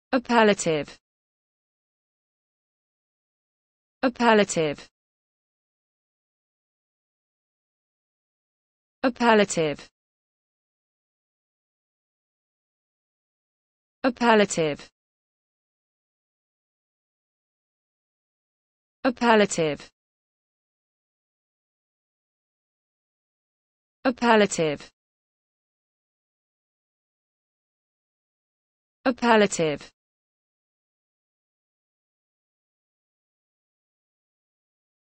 the correct pronunciation of appellative in English.
This video shows you how to pronunciation appellative in English.